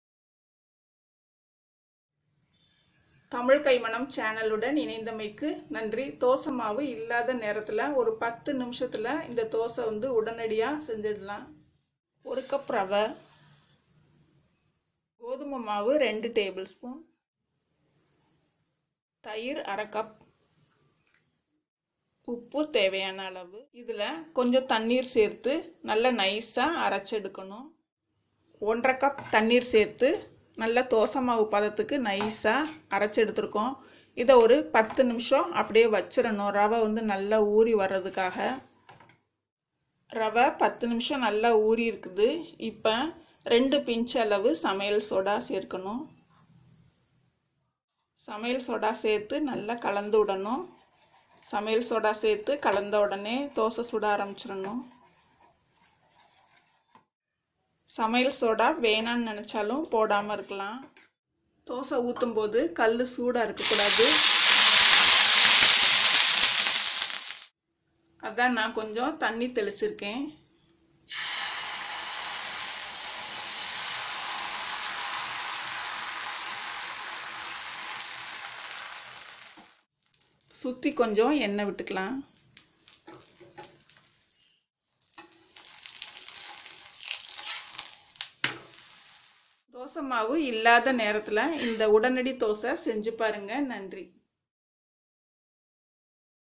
Instant Dosa Recipe / Udanadi Breakfast / Instant breakfast recipes Indian / Dosa without urad dal
தாேசை மாவு இல்லாத நேரத்தில், இந்த உடனடி தாேசை செய்திடலாம்.